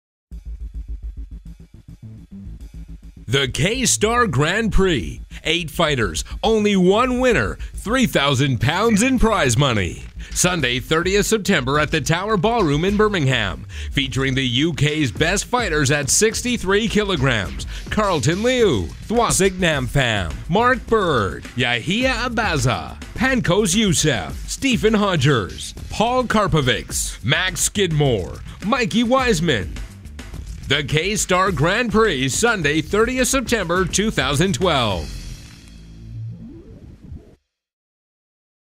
K-Star Grand Prix 2012
for up to date information about the K-Star Grand Prix 2012 , 30th September at the Tower Ballroom in Birmingham.
8 of the best fighters in the UK at 63kg battle it out for £3,000 in prize money.